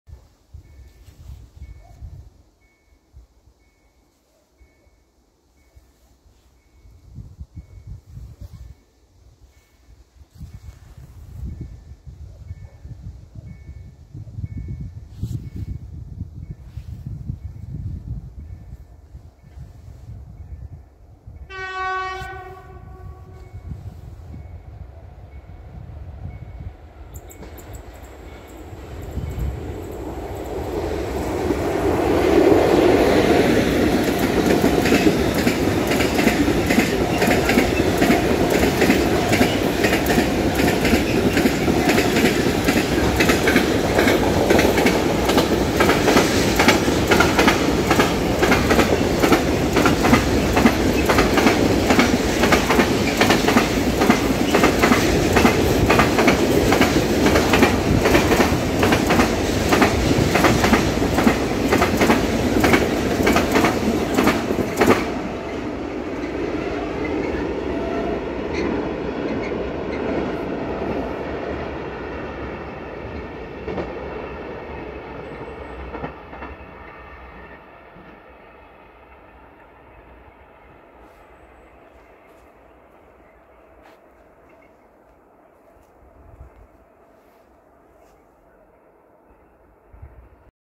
EU07-080 ECCO Rail
Kolejny sklad Ecco wjezdza na kanal, tym razem zlapany w Berezowie:)